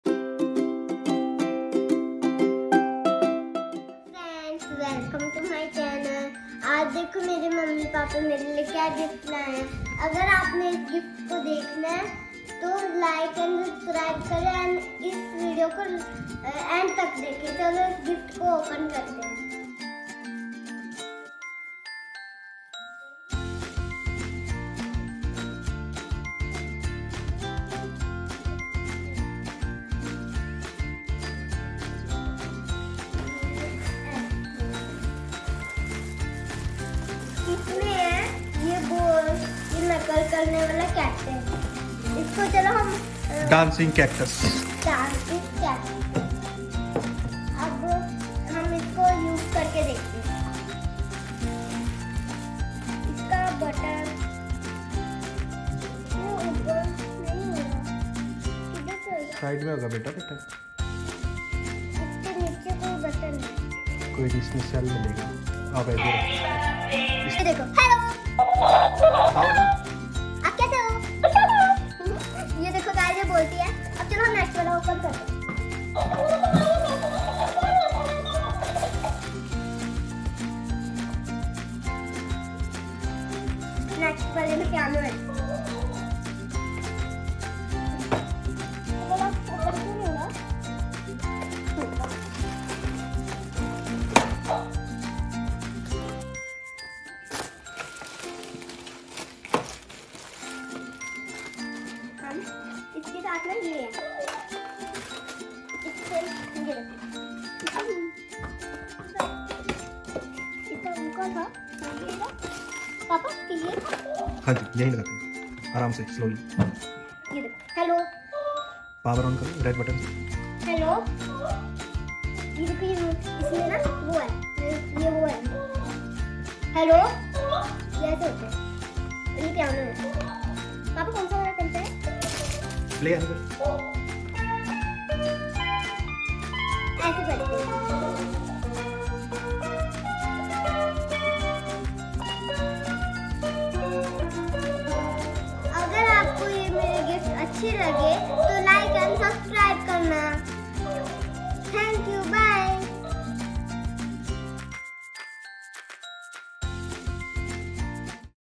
birthday gift opening
Sejal is very excited to show you her gifts. plz give blessings to kids. love you all ❤️